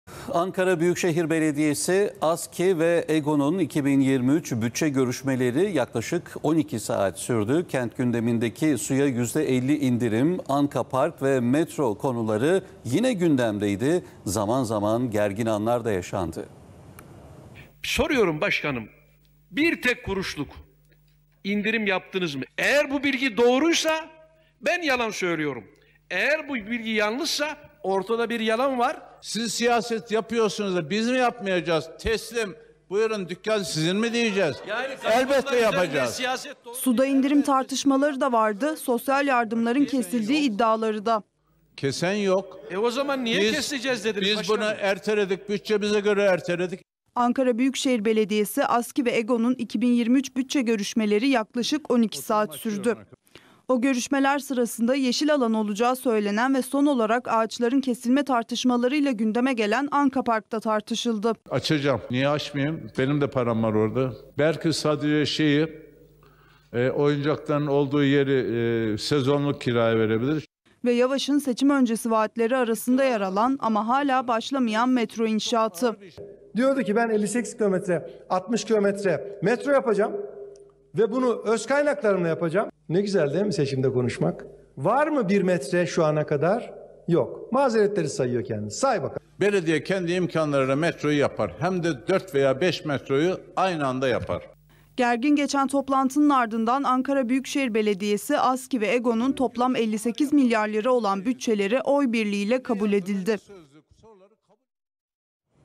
Ankara'da gergin bütçe görüşmeleri!
ABB, ASKİ ve EGO’nun ‘2023 bütçe görüşmeleri’ yaklaşık 12 saat sürdü. Kent gündemindeki ‘suya yüzde 50 indirim’, ‘Ankapark’ ve ‘metro’ konuları yine gündemdeydi. ABB Başkanı Mansur Yavaş bütçe sunumu yaparken muhalefet eleştirilerini yöneltti.